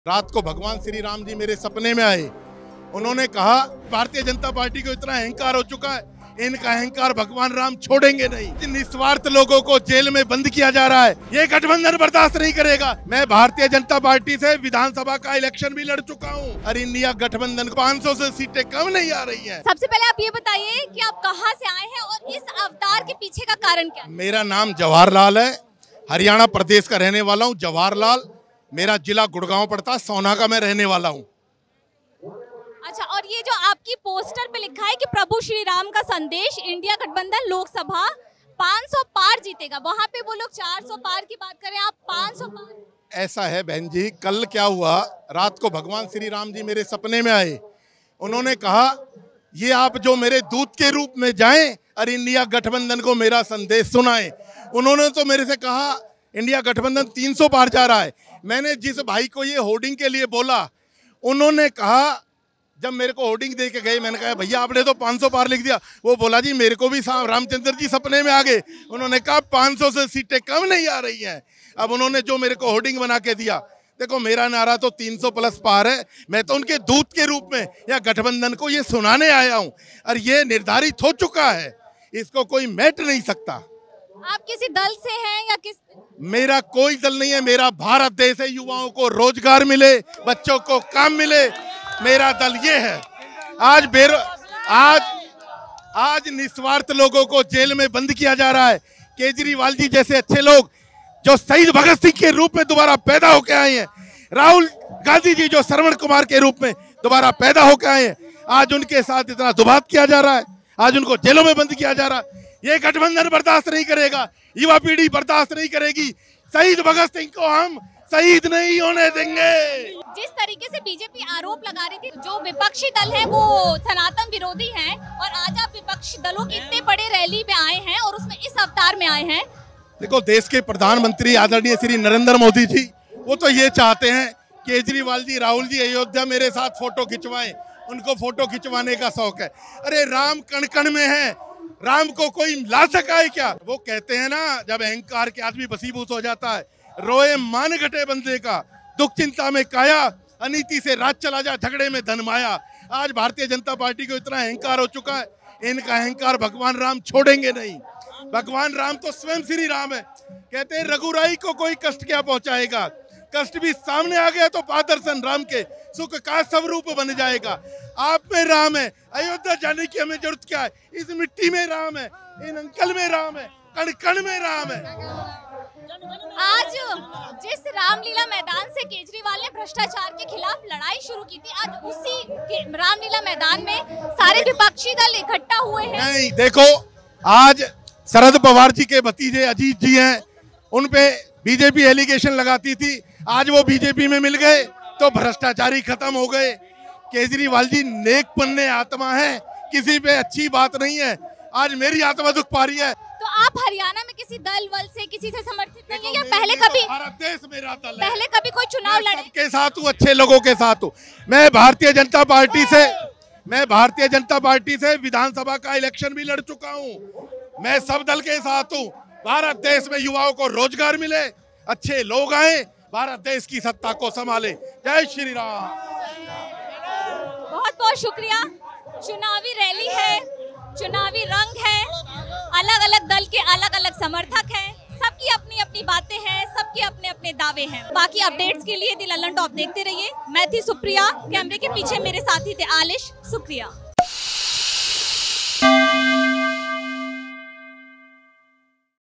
‘सपने में भगवान…’ India Alliance Rally में भगवान राम बनकर पहुंचे शख्स ने PM Modi, BJP पर क्या कहा?
Opposition alliance India Alliance held a mega rally on 31 March at Ramlila Maidan in Delhi. What do the people who came to this rally think about the Lok Sabha elections and Kejriwal's ED arrest? Lallantop's team talked to people on the ground regarding this.